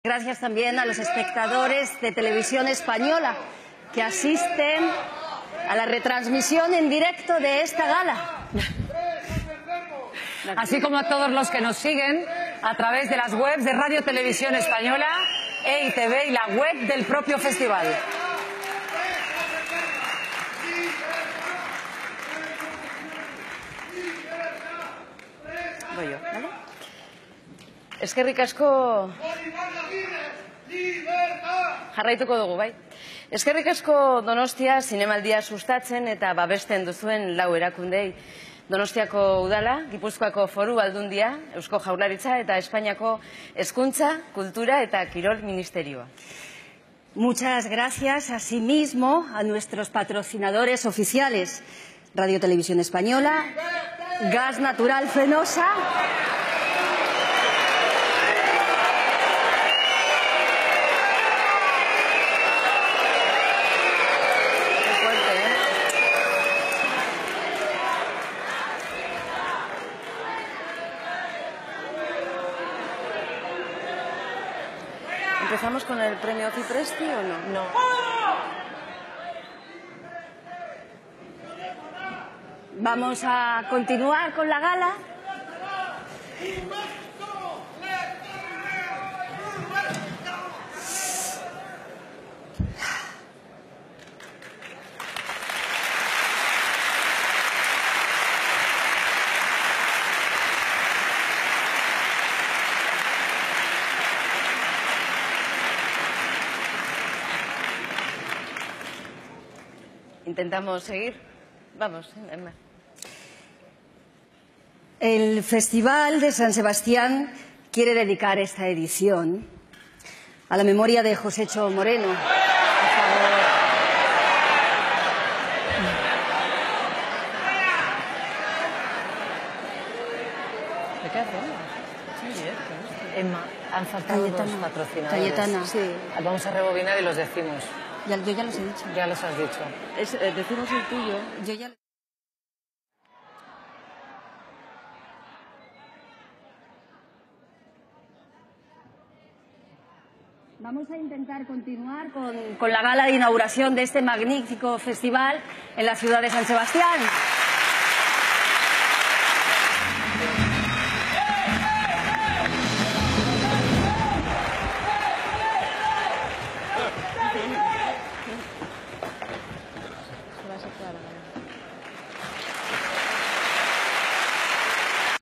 Protesta Zinemaldiaren inaugurazioan
Hainbat lagunek Zinemaldiko inaugurazio ekitaldia eten dute, Ibon Iparragirre presoaren egoera salatuz eta gaixo dauden presoen egoeraz ohartaraziz. Geroago, beste lagun bat irten da  agertokira, "Errausketarik ez" oihukatzen.